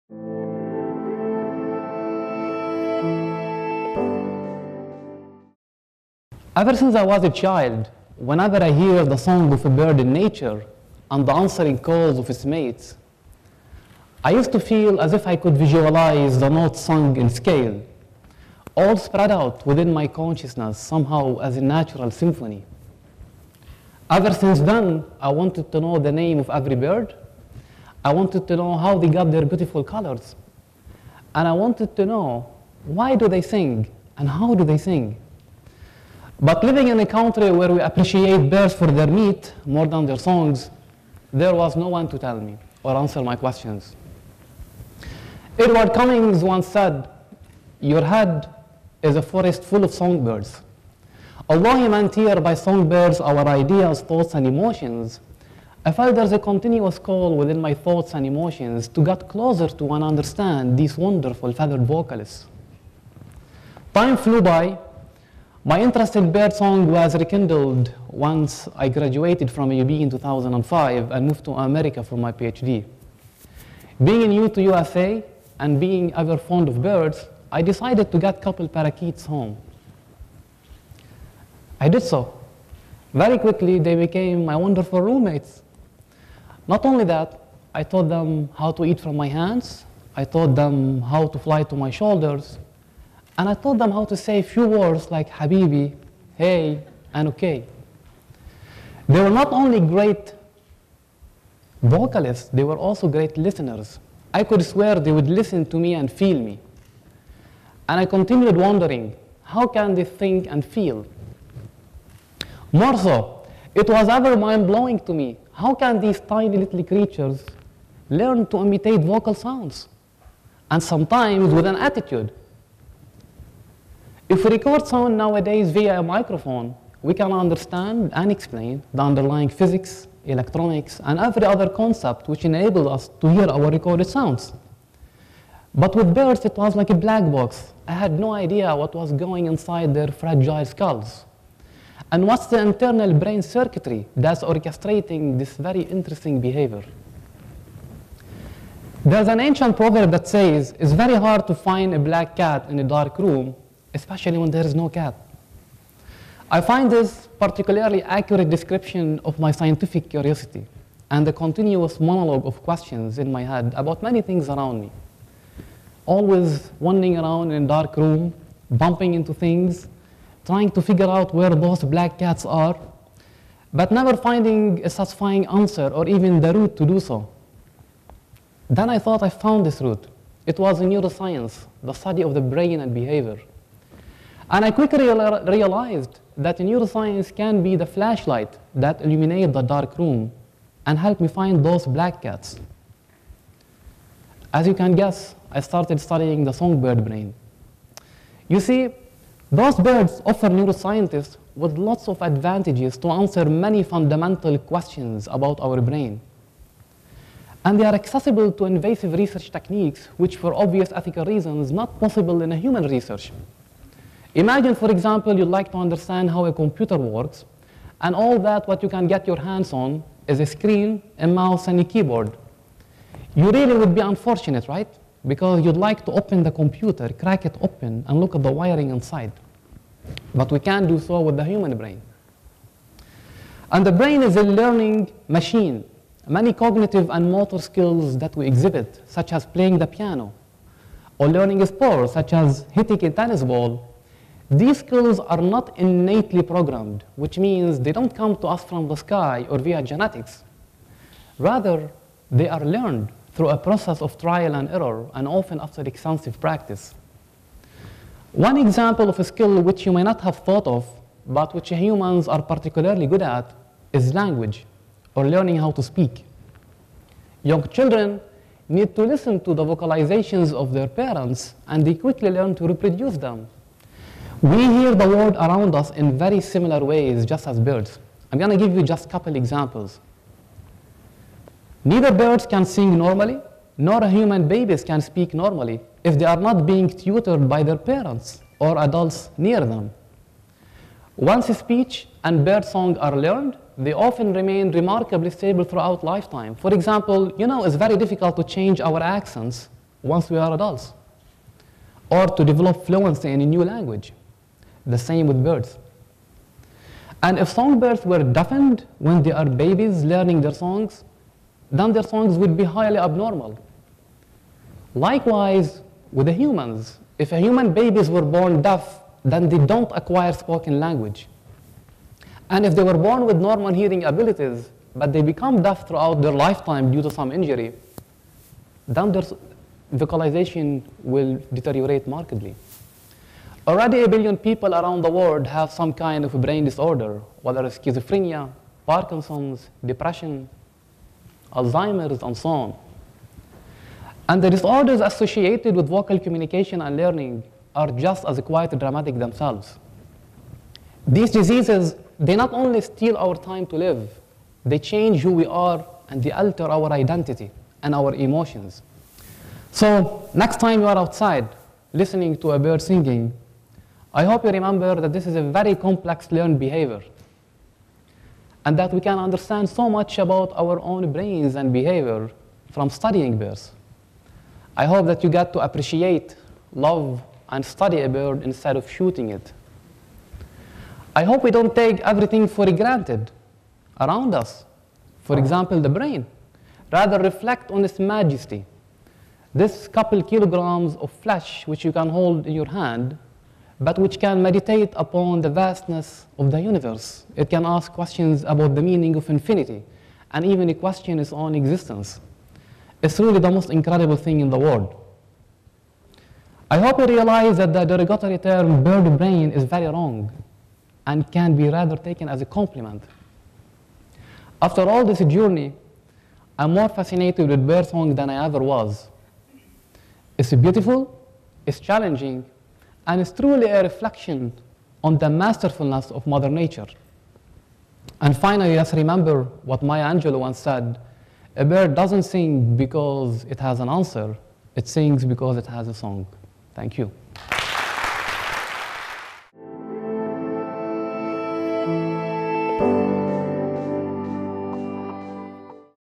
What the Songs of Birds Can Teach Us | Arij Daou | TEDxAUB
Passionate about birds and behavioral sciences, professor Arij Daou runs two labs between Chicago and Beirut which study brain conditions associated with disruptions in vocal learning, communication, and production. To approach this problem, he uses intracellular recordings from neurons in the brain in conjunction with neuropharmacology, immunohistochemistry, neuro-anatomy as well as mathematical modeling. Combining his passions he correlates the songs of birds to humans as a way to help vocal disabilities and stuttering. Arij Daou is a Lebanese neuroscientist and mathematician. An Assistant Professor at the Department of Biomedical Engineering at AUB.
His lab at AUB is focused on exploring neural sequence generation in the brain and the temporal precision that neurons in specific regions of the brain exhibit during complex motor behaviors. This talk was given at a TEDx event using the TED conference format but independently organized by a local community.